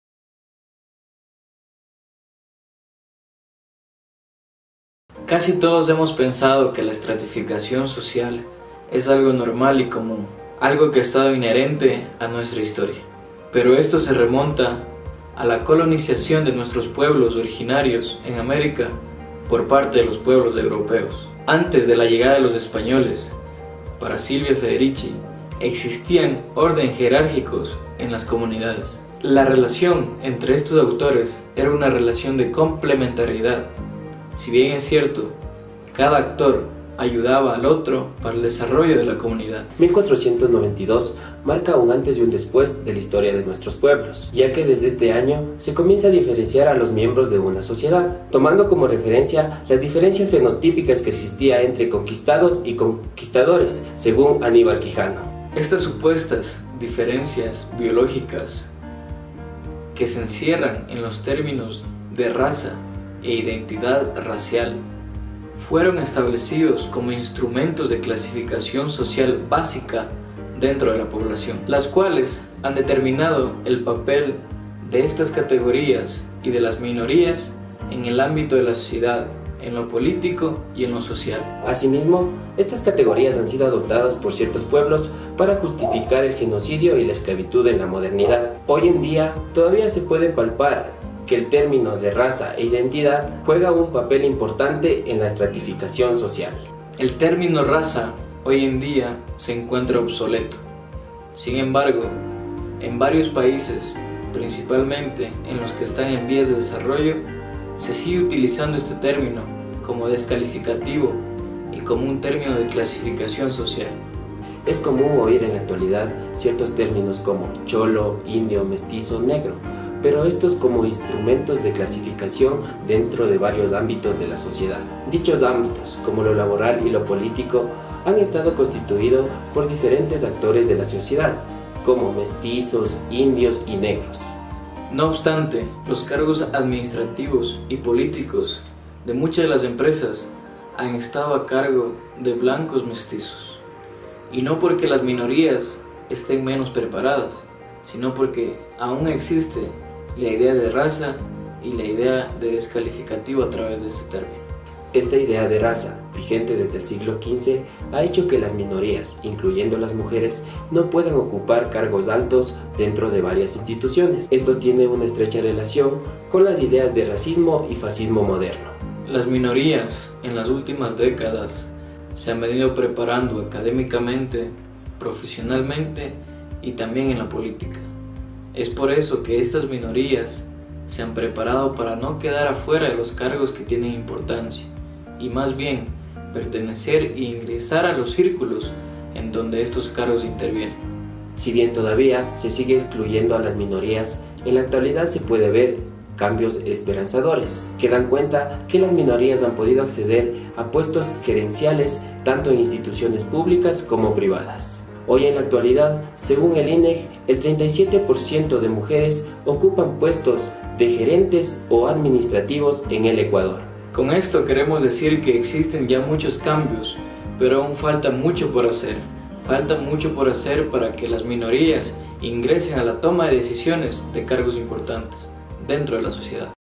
El concepto de raza como elemento decisorio en la jerarquización laboral
El concepto de raza como instrumento de clasificación social básica de la población (Quijano).
La raza como forma paradigmática de represión que sirve para justificar la esclavitud y el genocidio moderno  (Federici).